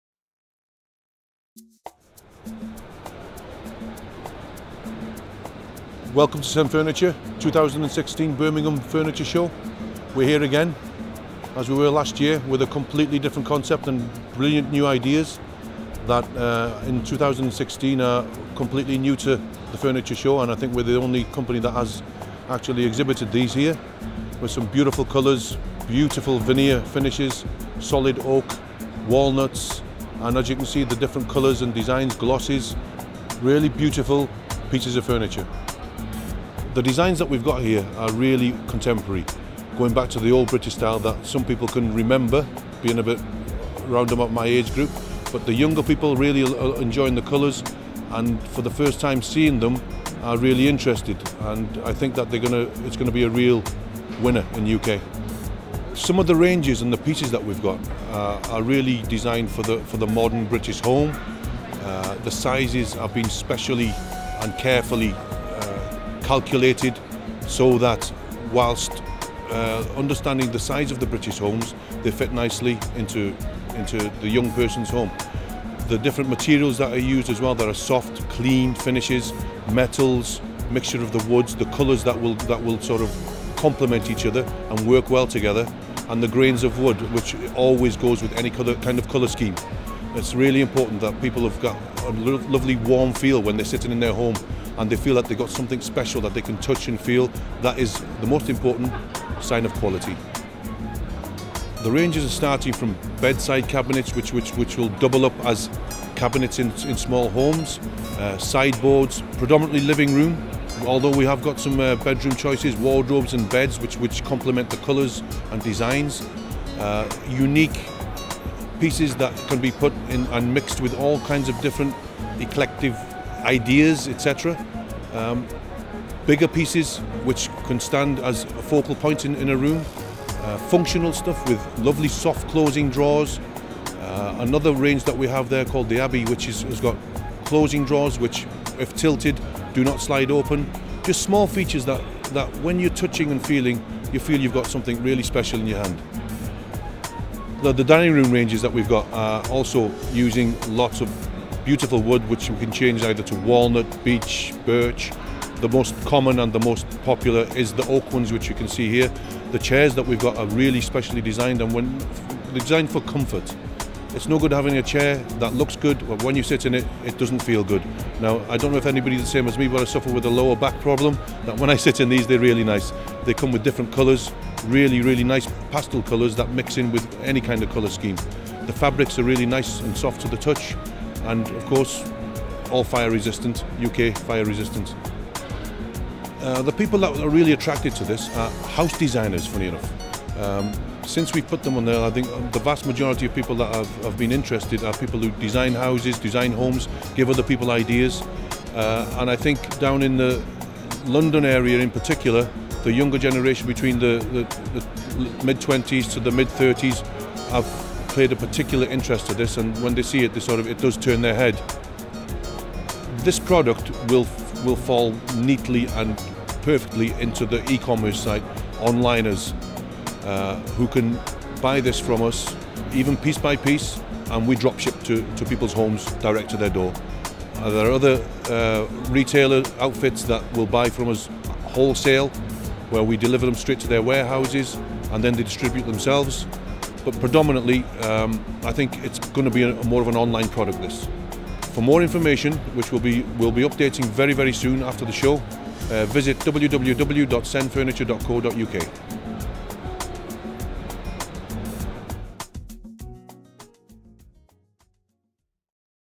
SEN Furniture
A brief catch up with Sen Furniture Director Yoan Wong to hear about some of the fantastic new ranges on offer from Sen Furniture for 2016.